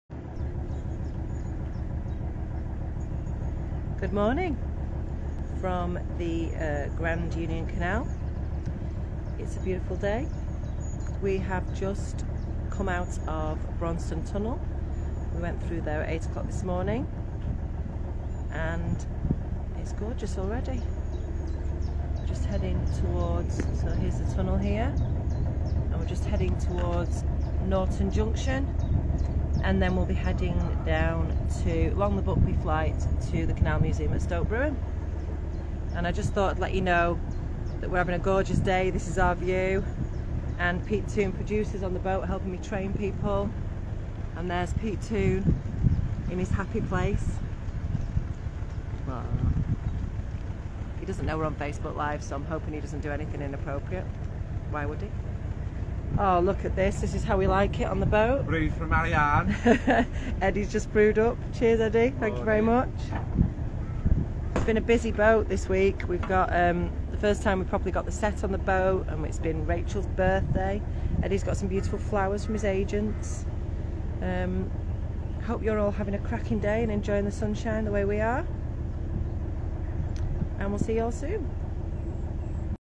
A morning commute - Mikron Theatre style!
Filmed just east of Braunston Tunnel on the Grand Union Canal 16th June 2023.

Marianne filming, Producer Pete steering and cast member Eddie brewing up!